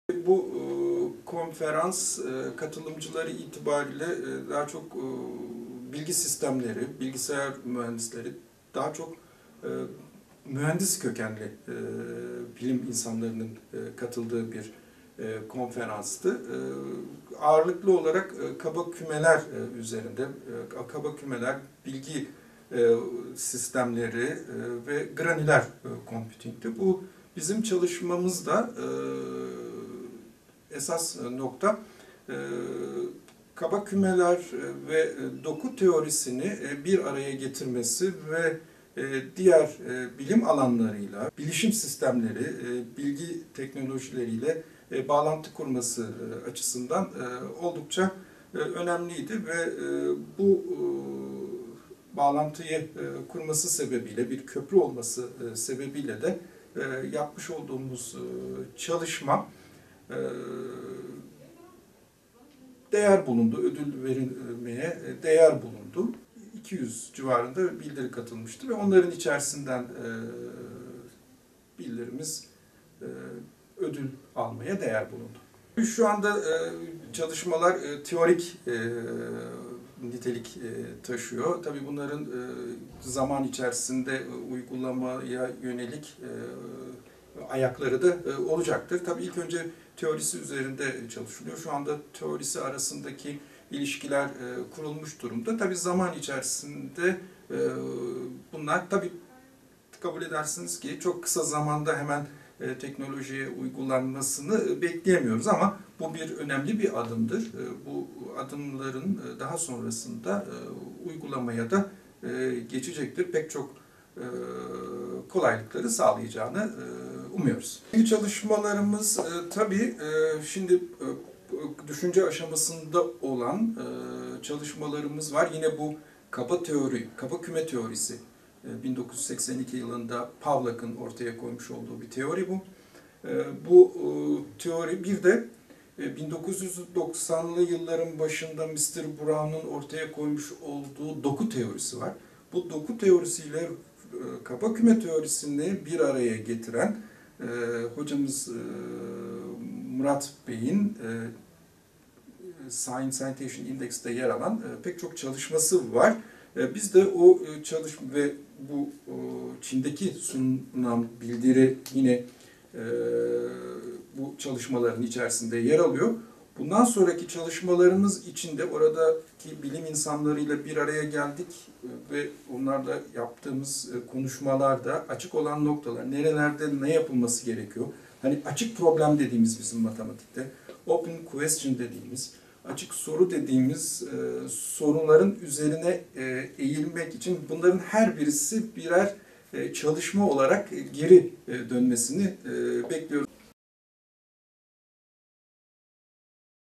Türk Bilim Adamları Çin’de “En İyi Makale” Ödülünü Aldı
Üniversitemiz ile Hacettepe Üniversitesinde görev yapan bilim adamları Çin Halk Cumhuriyeti’nin Tianjin Kentinde düzenlenen "The 2015 International Joint Conference on Rough Sets" konferansında sundukları bildiriyle "En İyi Makale" ödülünü aldı.